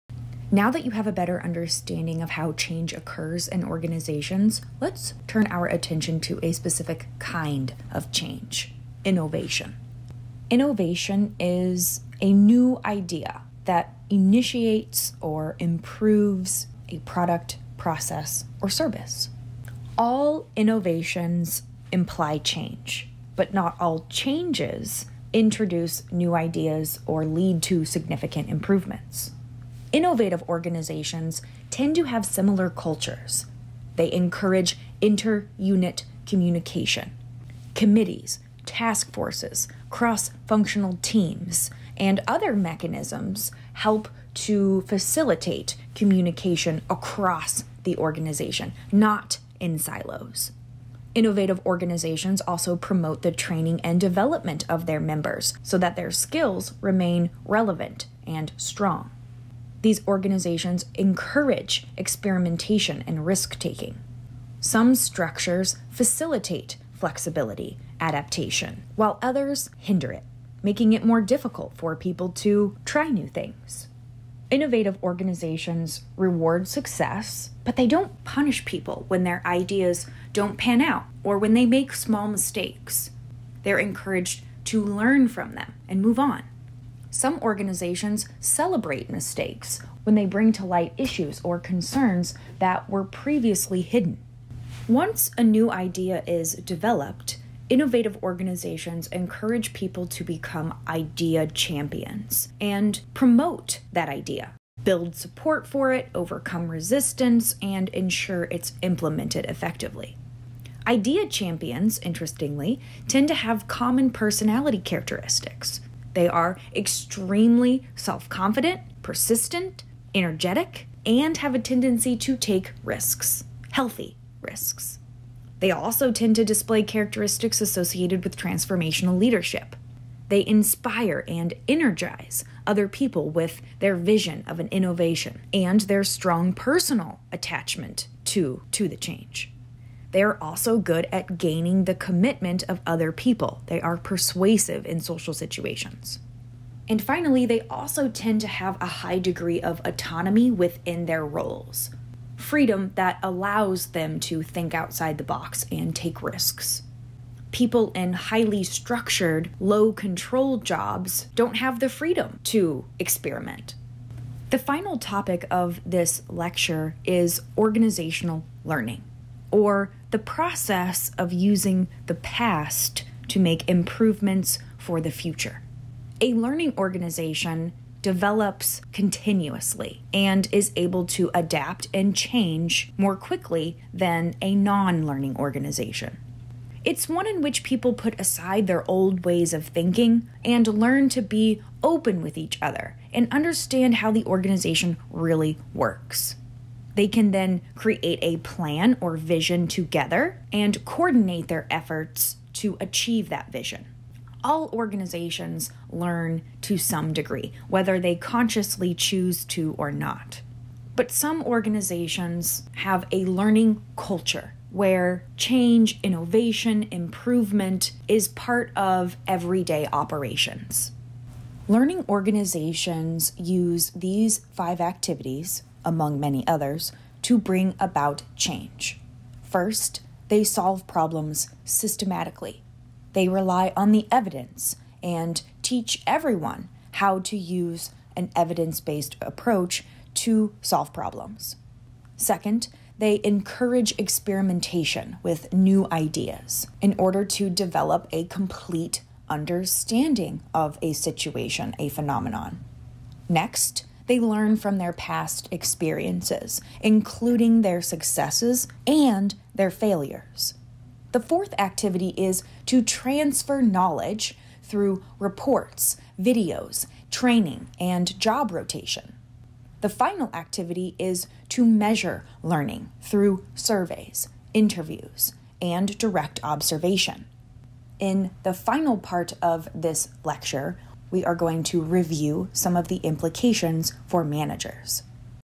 Organizational Psychology - Lecture 9 - Part 9 - Innovative Organizations and Learning Organizations
Feel free to post your questions & comments below.

Produced by Amy Quarton